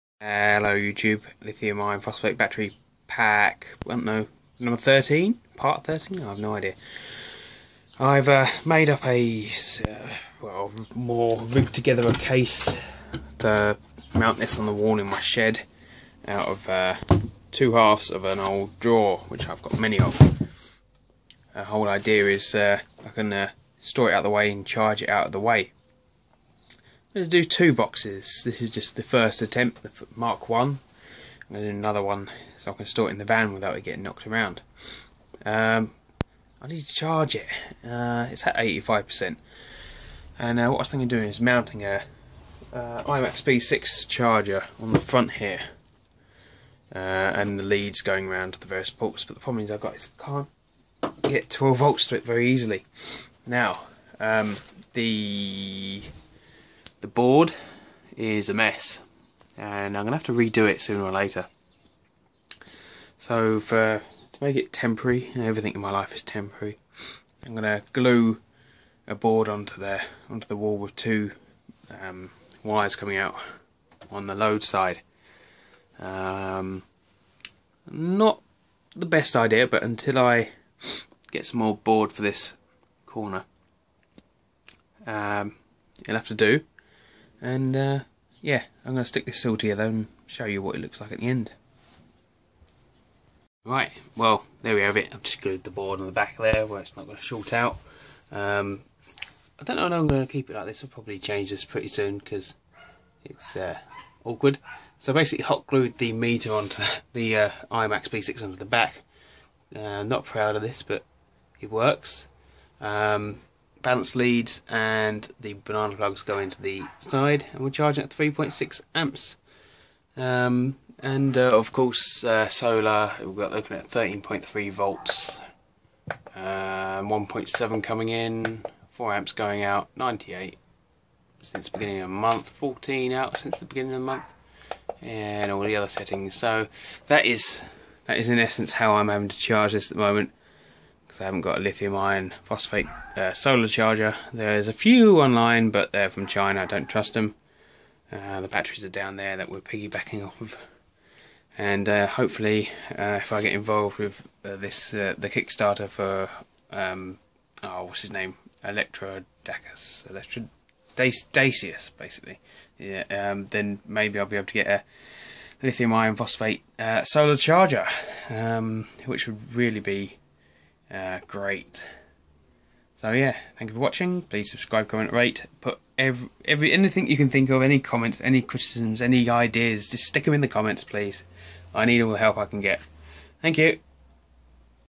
LIFEPO4 Part 13 - A place to charge
I made a shelf to contain the battery case out of 2 halfs of a woodern draw. I will end up doing two, so I can store the batteries in my van. I also rigged together an Imax B6 charger nearby so that I can charge this pack from ly lead acid battery bank.